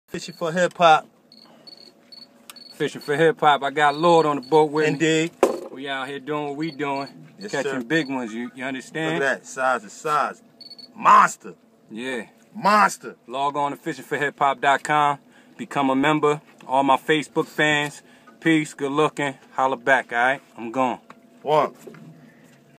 Monster Bass on Lake Lanier
Strip Bass Fishing For HipHop in Atlanta, Georgia. Fishing For HipHop Reality Fishing Show... visit FishingForHipHop.Com